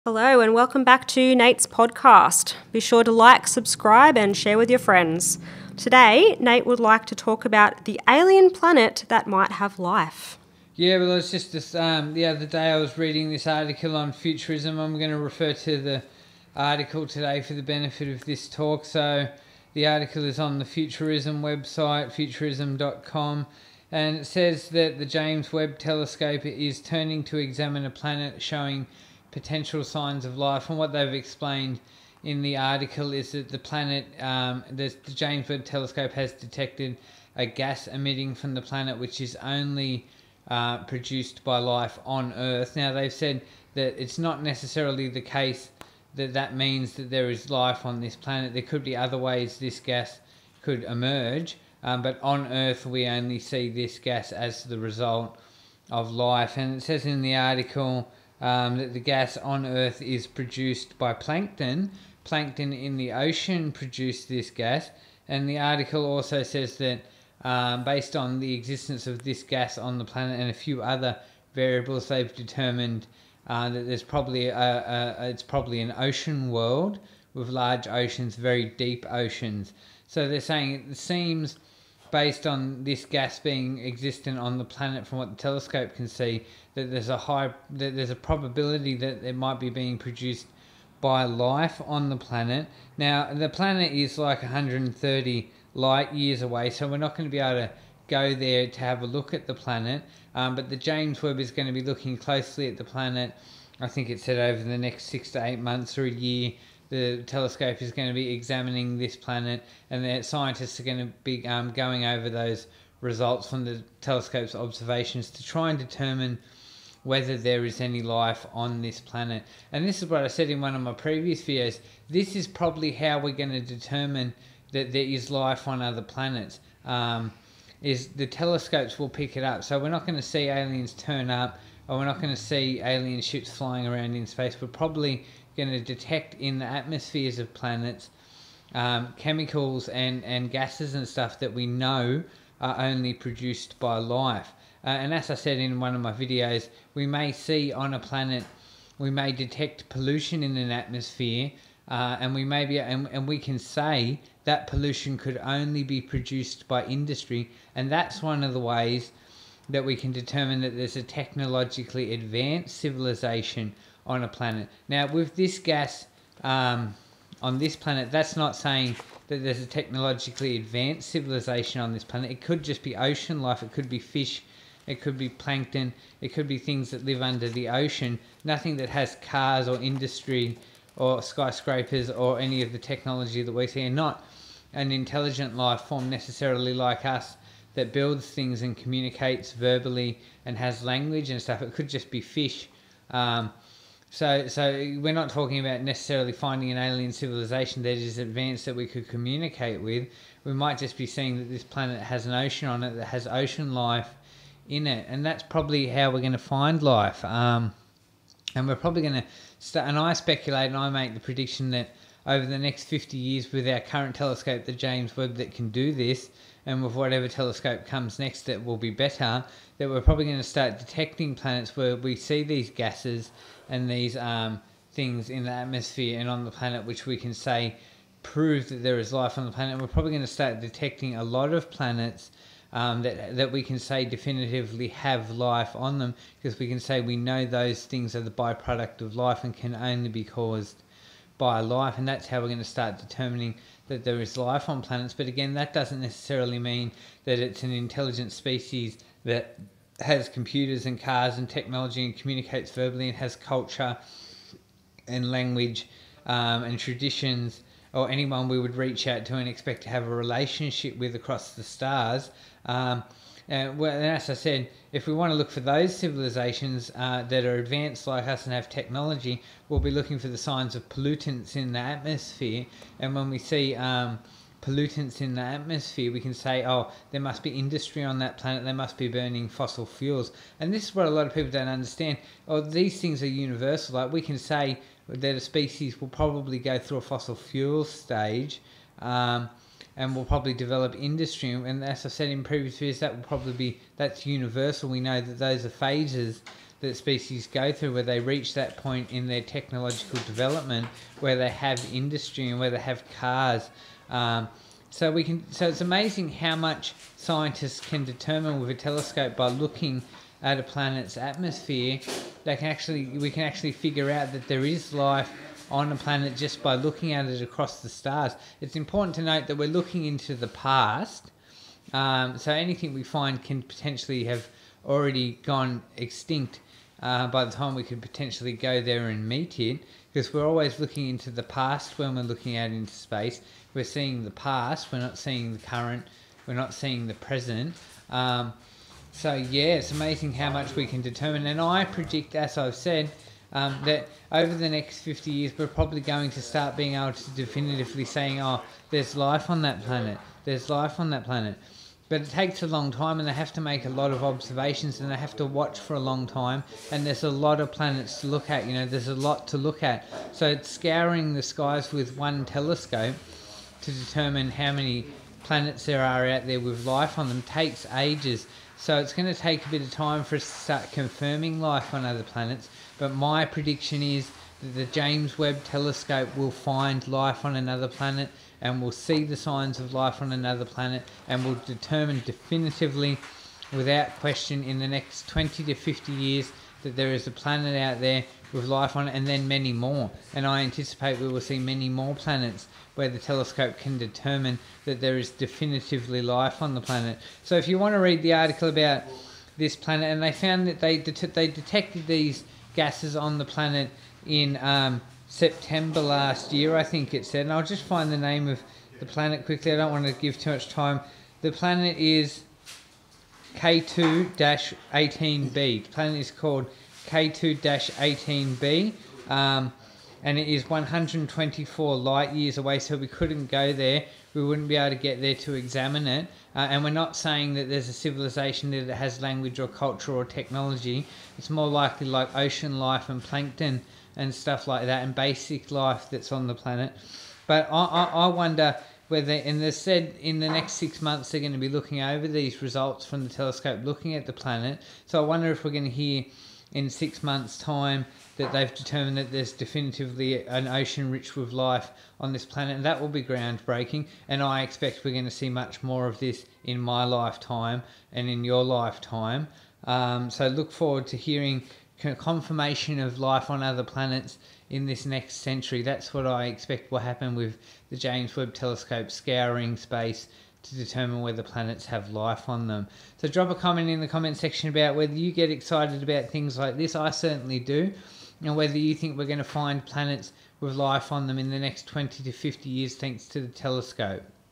Nathan Bell speaks about the planet K2-18b which may have life.
Interview with Nathan Bell about the planet K2-18b which may have life.